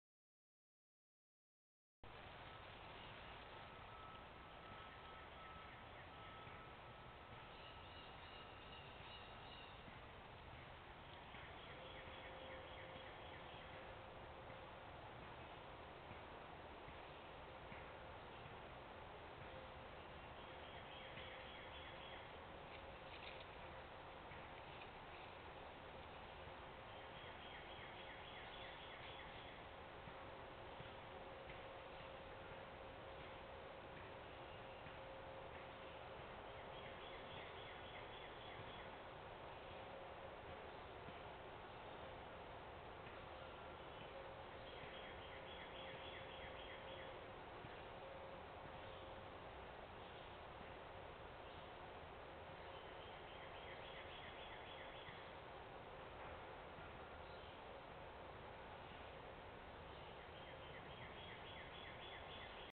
Capital Naturalist: Red Fox Eats a Plant
Red Foxes, like many canines, sometimes choose to eat and chew on leaves.